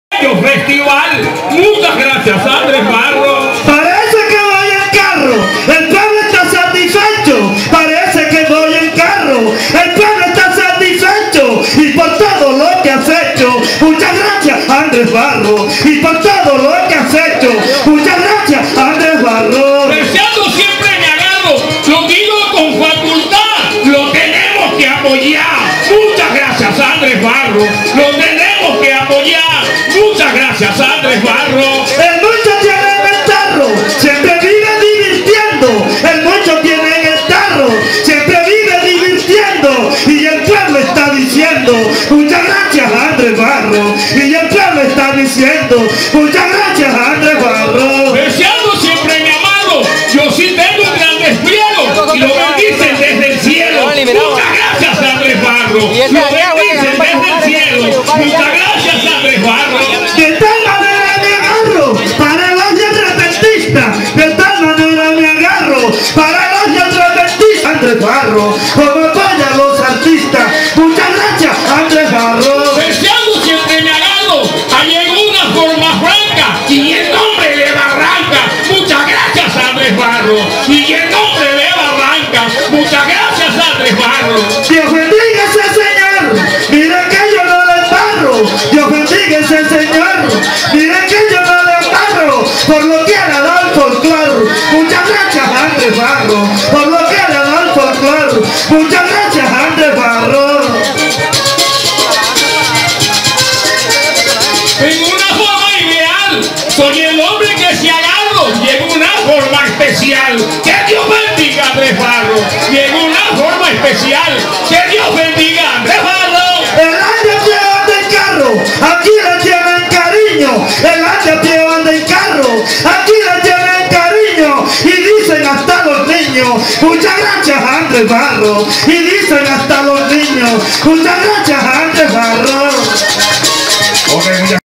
JULIO CARDENAS VS JOSE BORNACELY // PIQUERIA PIE FORZADO 2014
Festival nacional de la piqueria año 2014, pie forzado de dos grandes repentistas, pues ambos incluso son reyes vallenatos:

Jose Dolores Bornacely contra Julio Cardenas Guerrero